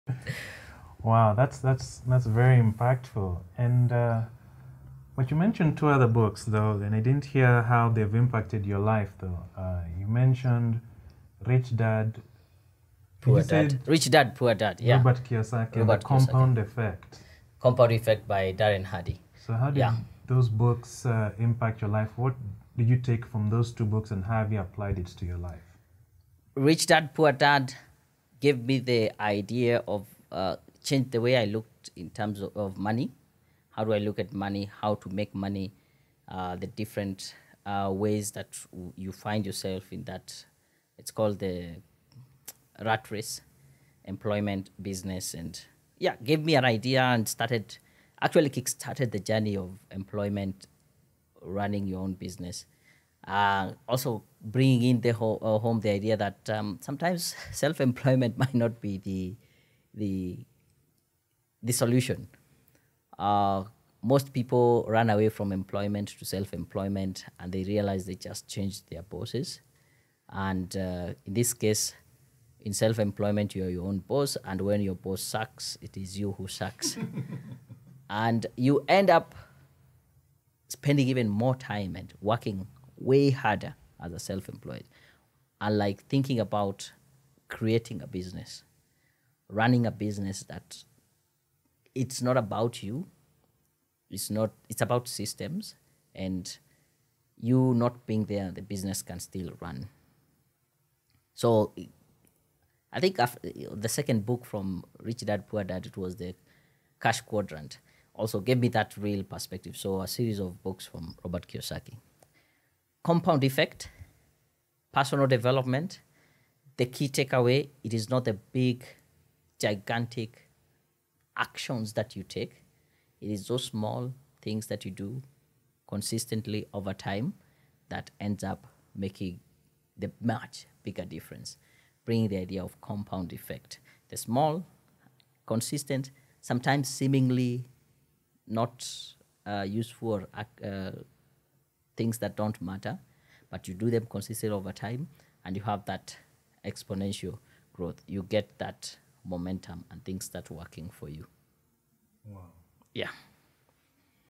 Daniel Mutuku: Books that have impacted your life | RFG06
Daniel talks about the books that have impacted his life and business